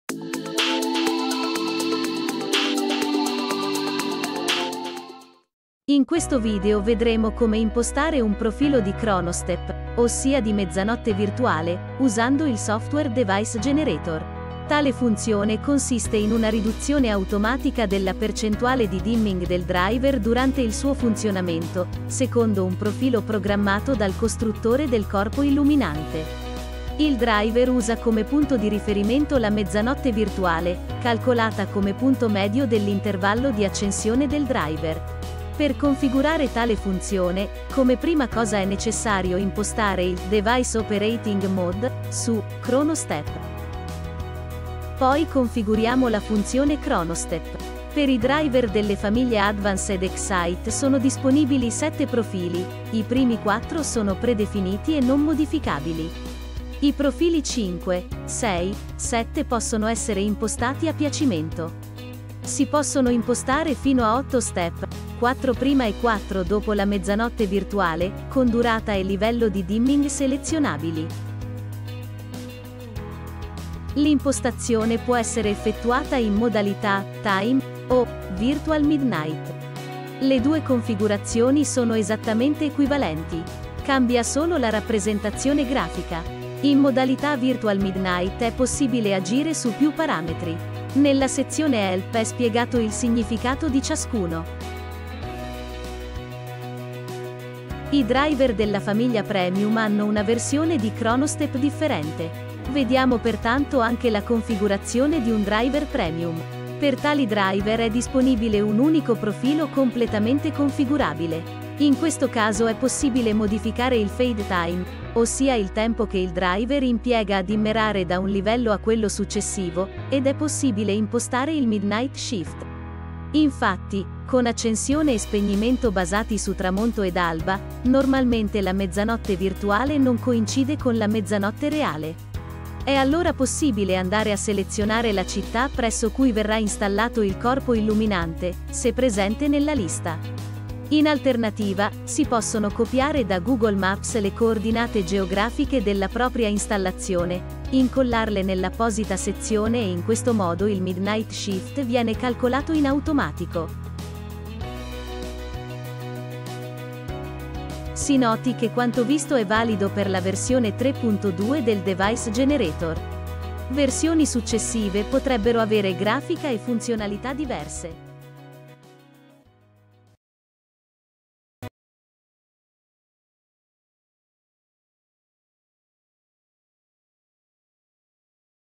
companionSUITE parte 06 – Come programmare la funzione chronoSTEP
Nel settore dell'illuminazione per esterni e dell'illuminazione stradale, spesso viene abbassato il livello di illuminazione durante le ore notturne per risparmiare energia. La funzione che permette di variare la percentuale di dimming durante il funzionamento del driver, comunemente nota come "mezzanotte virtuale", è da Tridonic denominata chronoSTEP.
Il driver non è dotato di un clock interno, ma misura automaticamente il tempo di accensione, che è in genere l'intervallo tra il tramonto e l'alba.
Il punto medio di questo tempo di accensione misurato è chiamato mezzanotte virtuale.
Vedremo in questo video come configurare la funzione chronoSTEP all'interno del deviceGENERATOR.